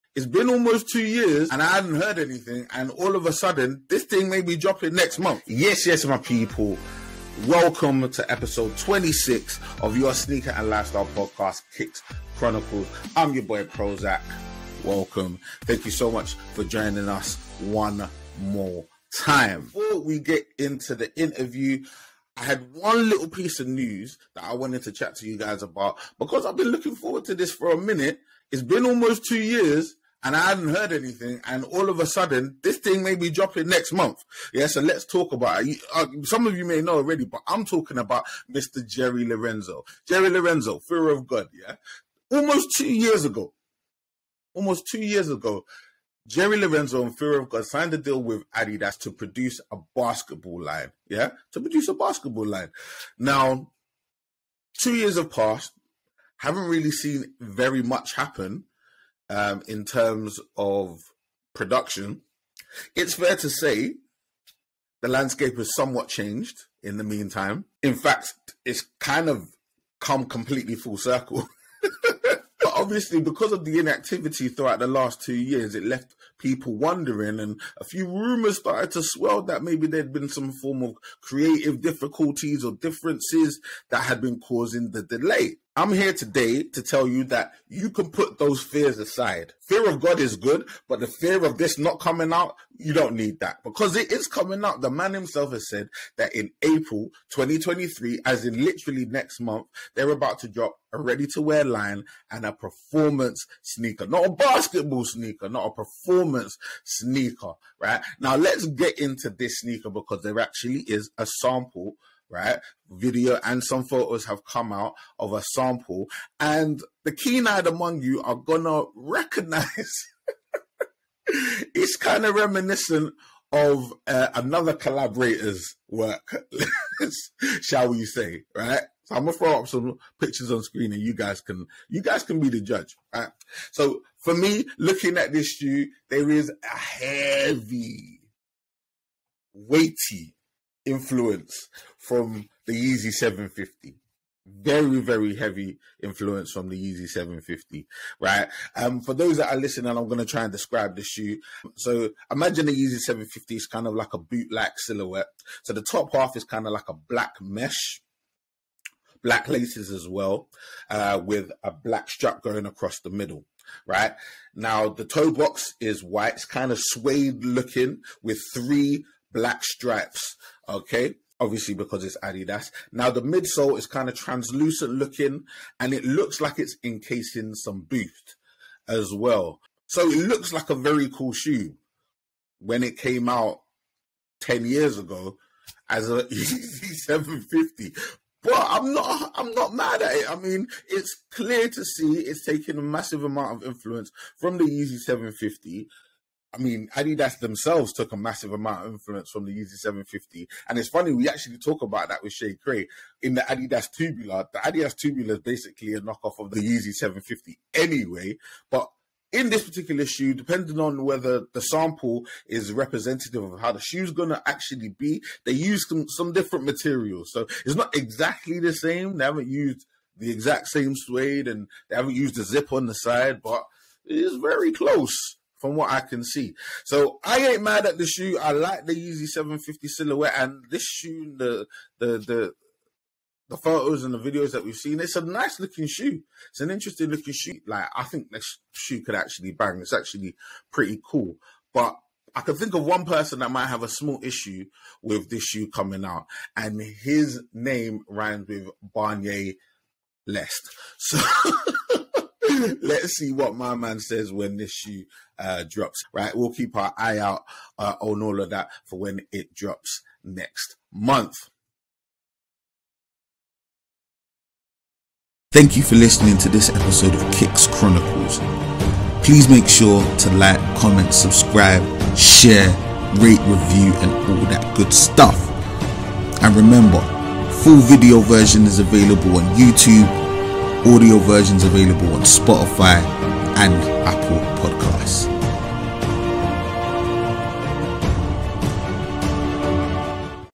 IS THE FEAR OF GOD x ADIDAS Sneaker is a YEEZY 750 REP? | RTW Line Drops April | KICKS KHRONICLES
Fear of God x Adidas after 2 years are rumoured to be dropping a RTW line in April 2023 but, is their new performance sneaker a Yeezy rip off???

Want to start reselling, but don’t know how - join CREP CHIEF NOTIFY

Interesting in Podcasting - Kicks Khronicle Podcast recorded using platform Riverside.fm. For more information shorturl.at/lBHU5